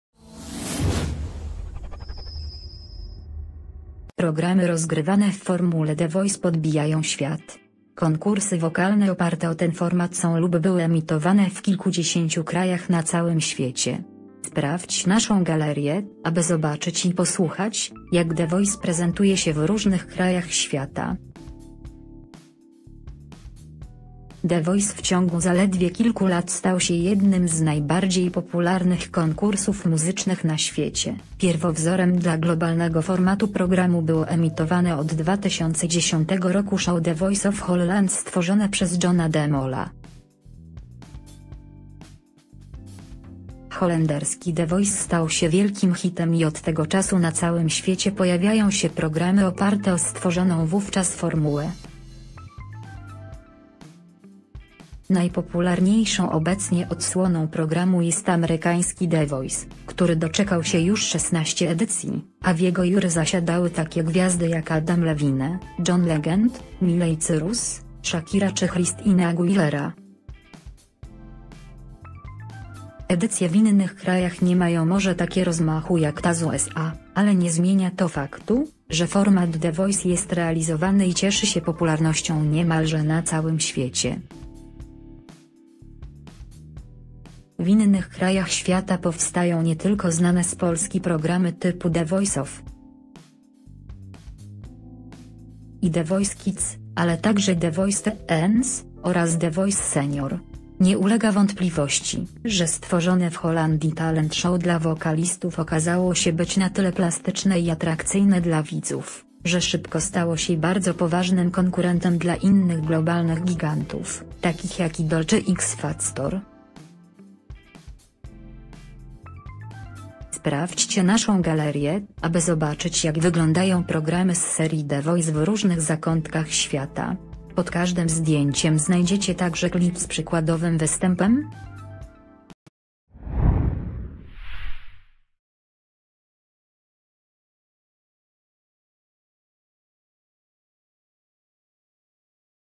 BREAKING NEWS: "The Voice" na świecie. Jak wygląda popularny muzyczny show w innych krajach?
Programy rozgrywane w formule "The Voice" podbijają świat! Konkursy wokalne oparte o ten format są lub były emitowane w kilkudziesięciu krajach na całym świecie! Sprawdź naszą galerię, aby zobaczyć i